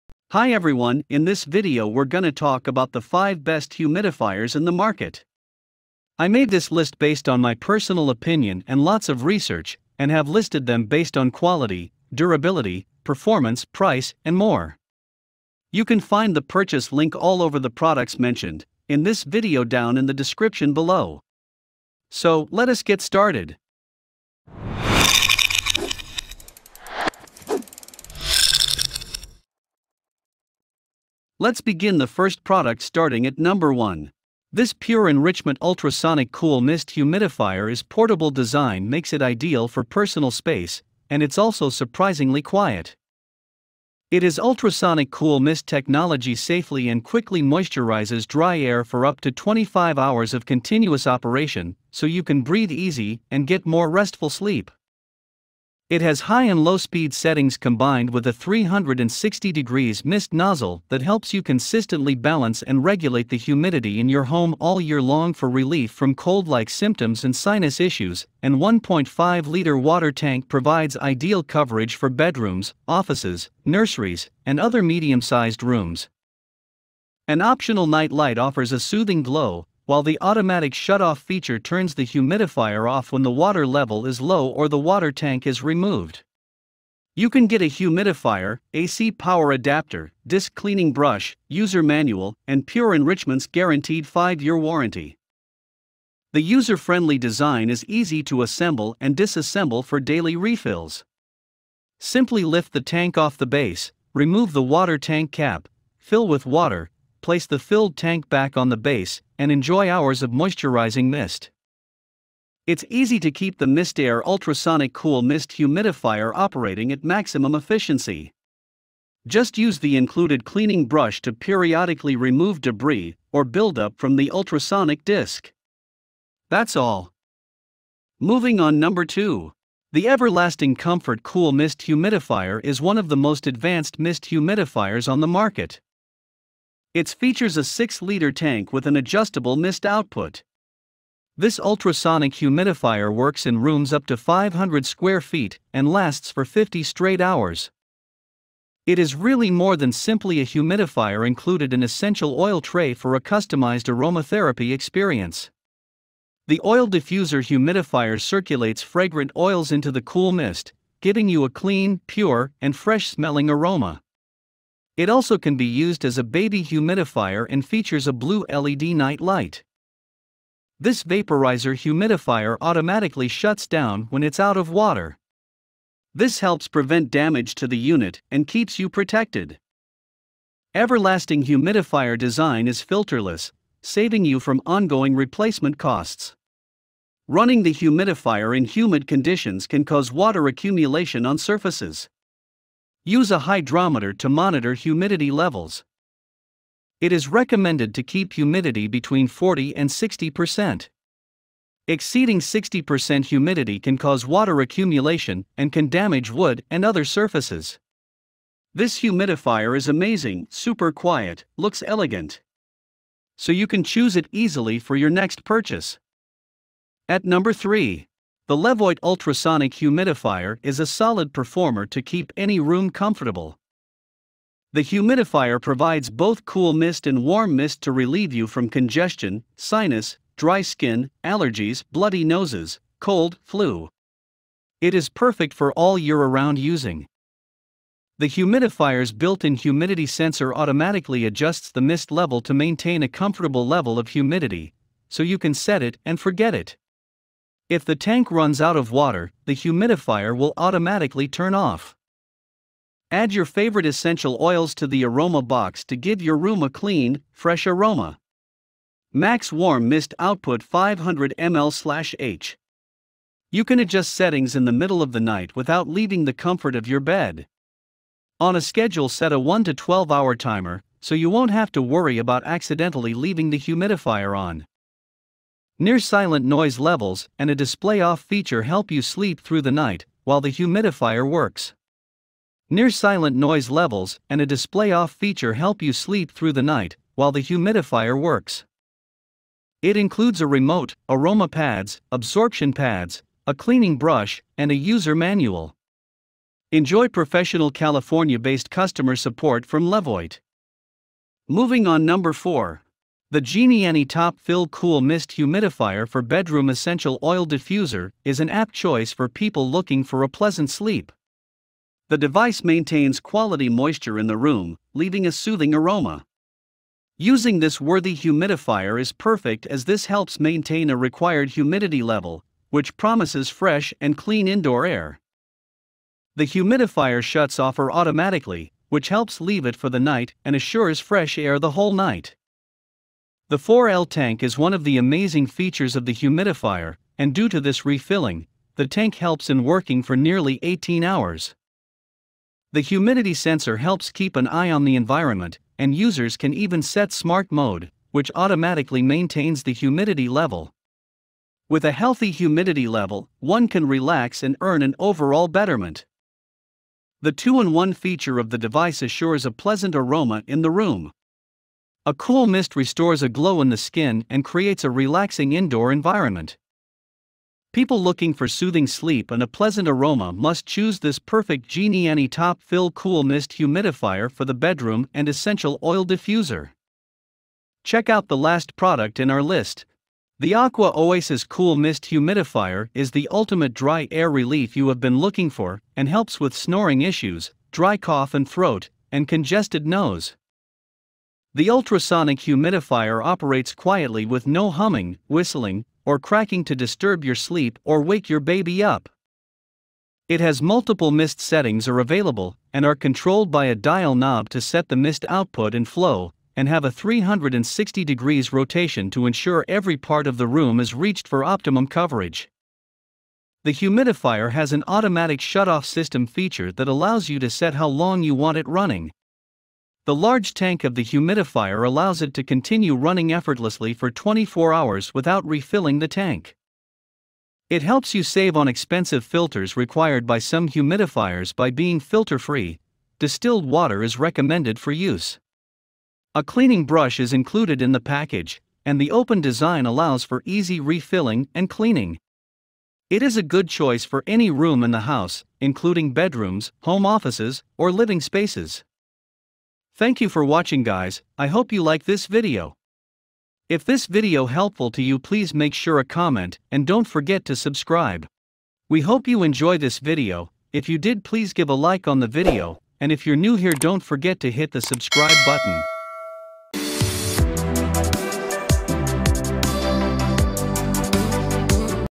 Top 5 Best Humidifiers in 2023 [Review & Buyers Guide]
In this video, we are going to reveal the Best Humidifiers on Amazon of 2023

1. Pure Enrichment Ultrasonic Cool Mist Humidifier

2. Everlasting Comfort Cool Mist Humidifier

3. LEVOIT Humidifiers for Large Room Bedroom

4. GENIANI Top Fill Cool Mist Humidifiers

5. AquaOasis Cool Mist Humidifier

During the winter months, as the mixture of cold air and overheated homes dries out the skin humidifiers can boost much-needed moisture, releasing water vapor into the air. That moisture helps alleviate dry skin, chapped lips, nose bleeds, and other cold-weather conditions.

Although fewer somebody thinks to use them during the summer months, humidifiers aren’t just for the wintertime, described Jessie Cheung, MD, a board-certified dermatologist. “Moist air helps slow down the release of microbes and can combat the dry air from summer air conditioning,” she said.

It also allows for more comfortable breathing if you have blocked sinuses or respiratory infection and helps to prevent sensitive skin from drying out.” Therefore, spending in a humidifier gives you year-round benefits.

Sometimes, we use some promotional and educational footage in our content.